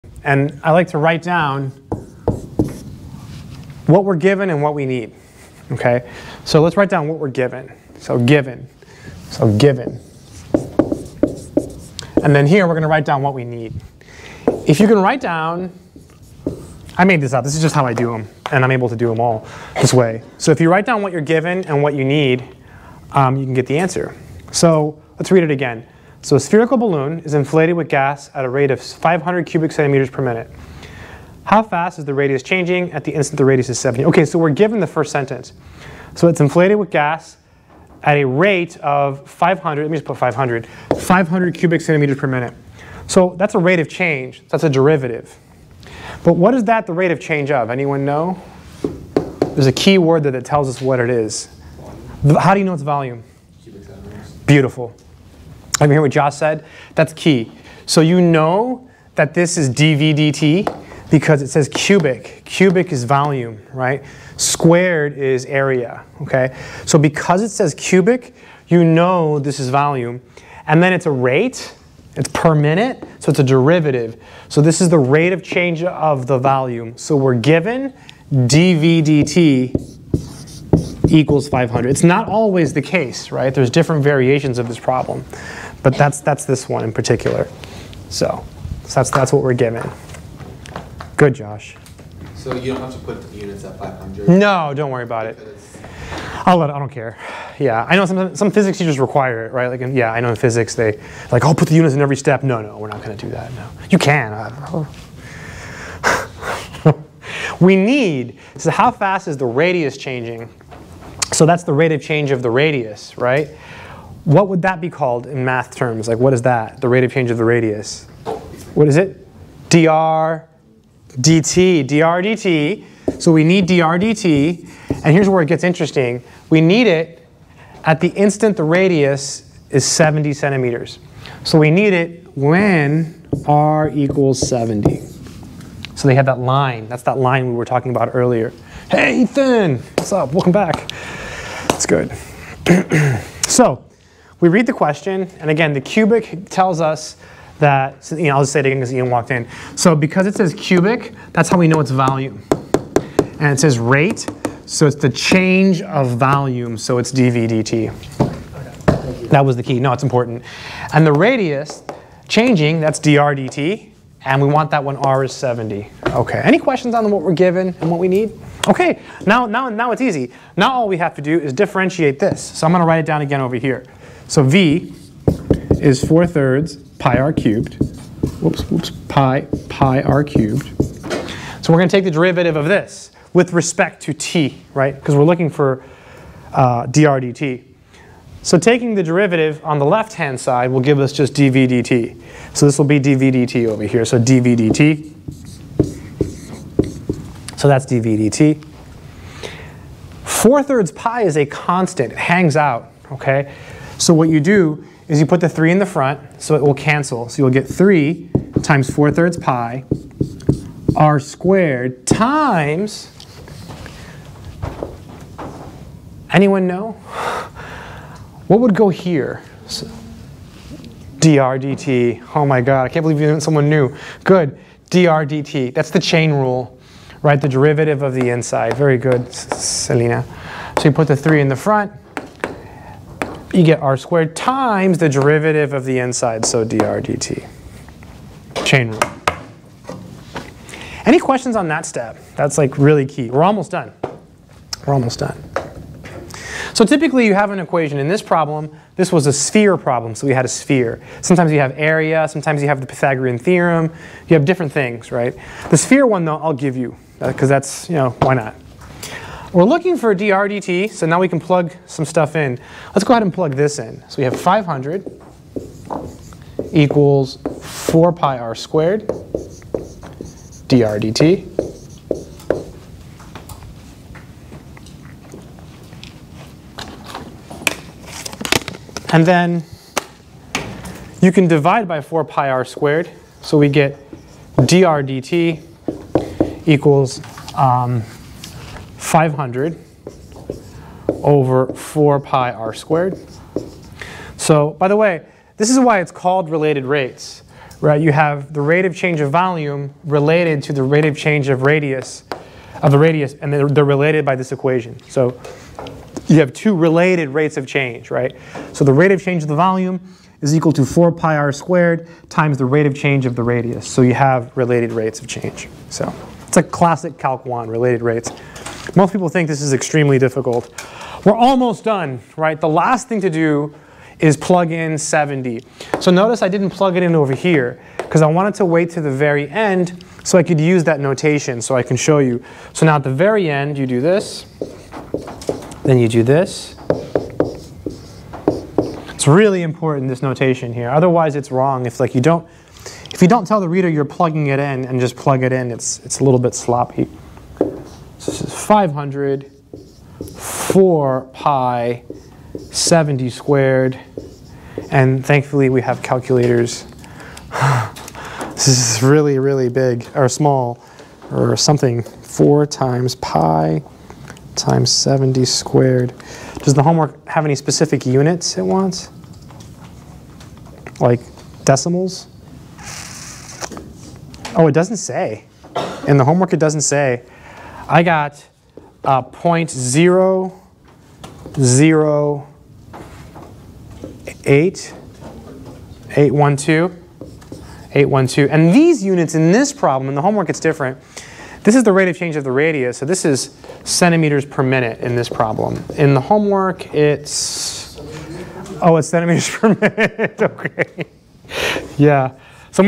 How Fast is the Radius Changing Spherical Balloon Related Rates
In this video I do an example of a related rates word problem from calculus. The question is to determine how fast the radius is changing. This is the balloon problem.